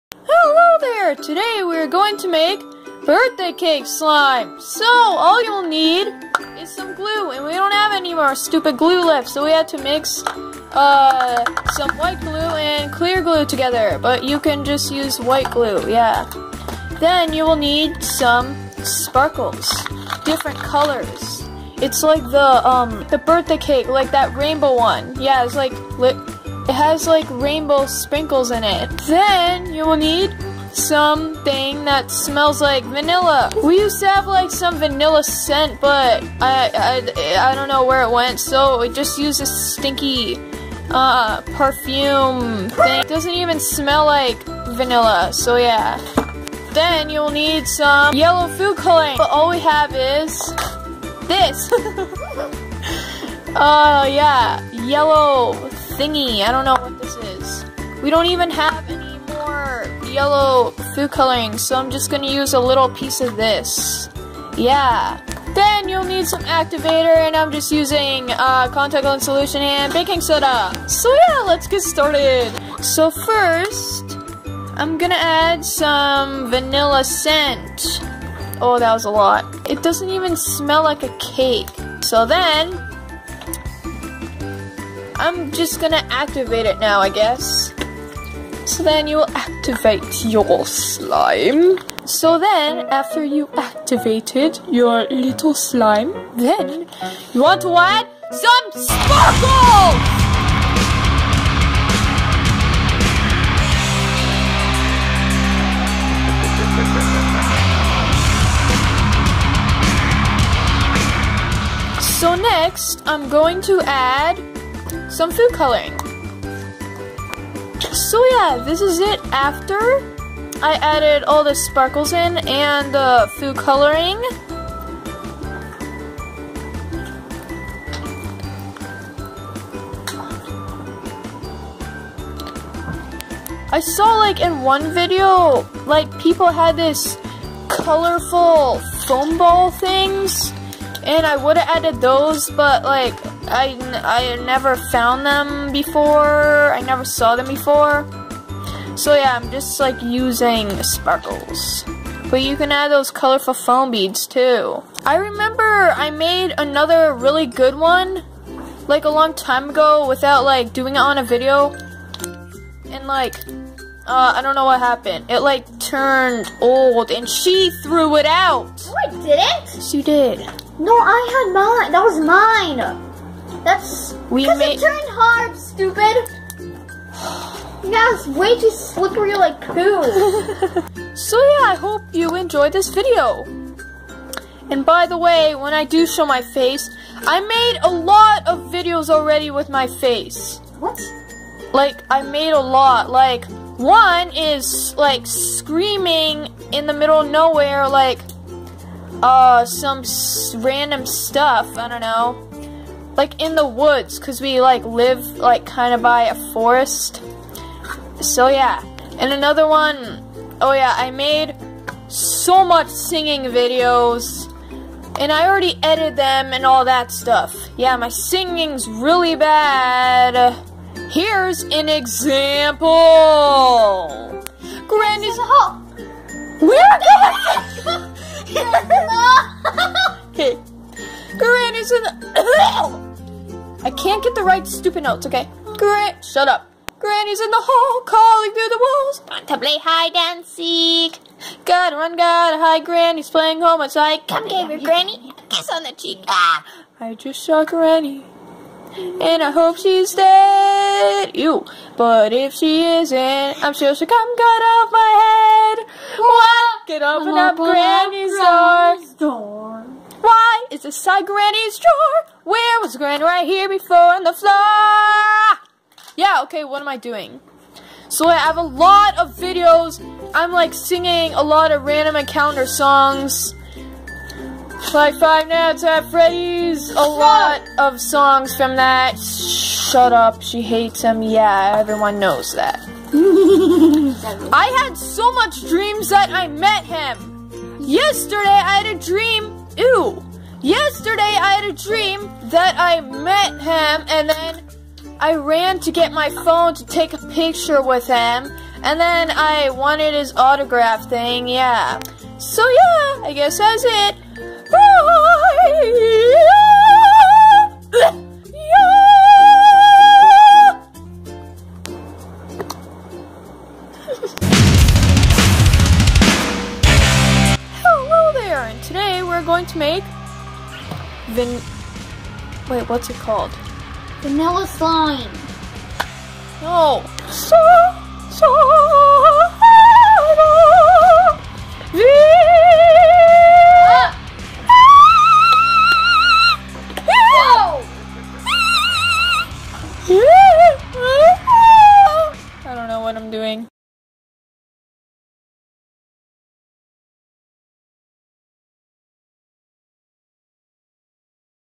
How To Make Birthday Cake Slime
It looks so good. It looks like a real birthday cake. I want to eat it.

P.S. Happy Birthday Markiplier!!! ❤️
His channel if you don't know who he is:

Also RIP Miranda